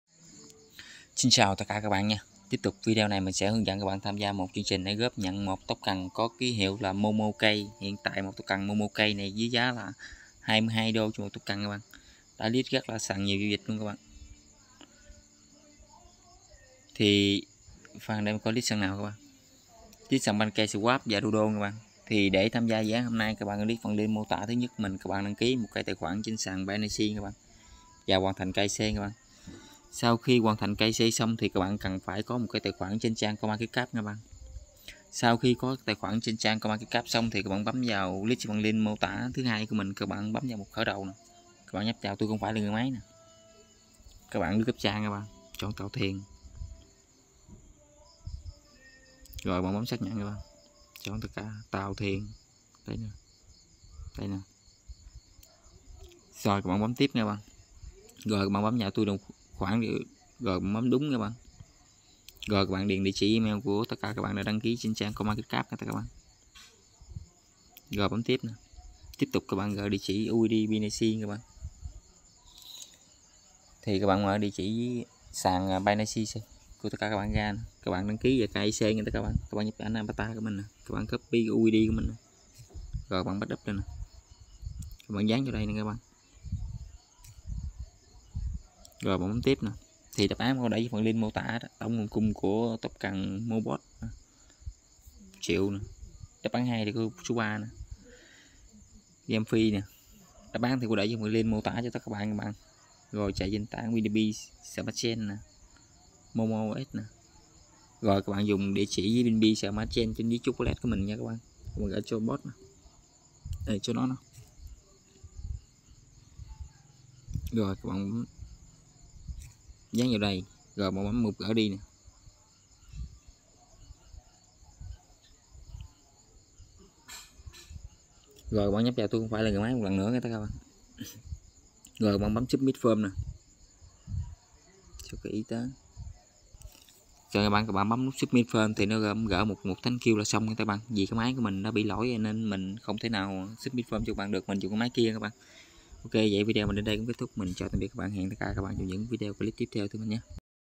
hướng dẫn nhận 1 token KEY ~22$ (từ sự kiện sàn binace) kèo ngon ăn chắc 💯
Links vào sự kiện
Đáp án đúng
Đáp án
:1. 1,000,000,000
 2. 3
3. GameFi
4. Combining cross chain and cross platform...
5. Binance Smart Chain
6. MOMOs